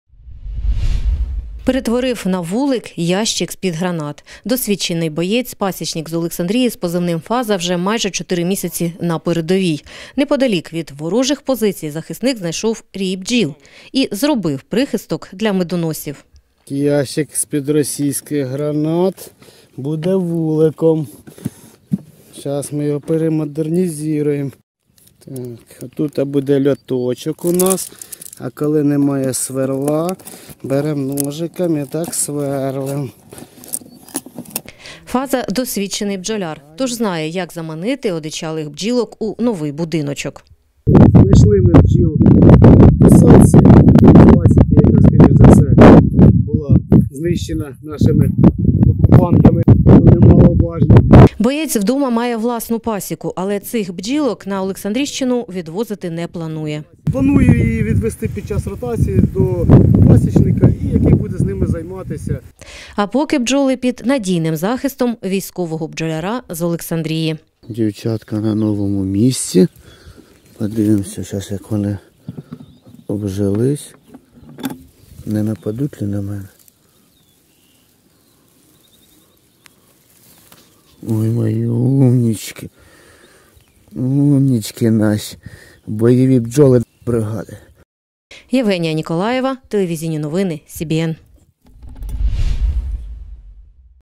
Ящик з-під гранат на вулик перетворив на передовій боєць з Олександрії | телеканал Вітер
Перетворив на вулик ящик з-під гранат. Досвідчений боєць, пасічник з Олександрії з позивним Фаза, вже майже чотири місяці на передовій. Неподалік від ворожих позицій захисник знайшов рій бджіл. Як досвідчений бджоляр зробив прихисток для медоносів - дивіться у сюжеті.